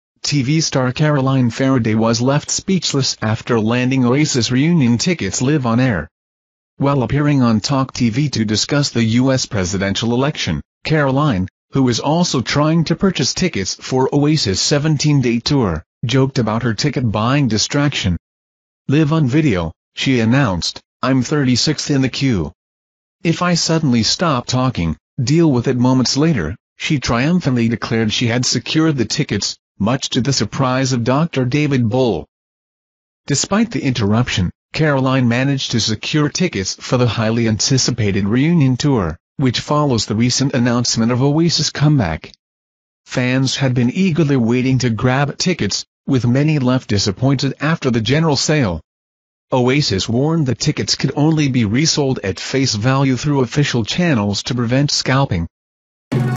TV star Caroline Feraday left completely speechless as she lands Oasis tickets live on air
Watch the unforgettable moment when TV star Caroline Feraday scored Oasis reunion tour tickets live on air! While discussing the US presidential election on TalkTV, Caroline hilariously multitasked by trying to grab tickets for the highly anticipated 17-date tour. Despite being 36th in the queue, she triumphantly announced she secured the tickets, leaving viewers and her co-host, Dr. David Bull, amazed. With Oasis' comeback tour creating a frenzy, this live ticket-buying moment is one for the books. Catch all the excitement and find out how Caroline managed to get her hands on these sought-after tickets!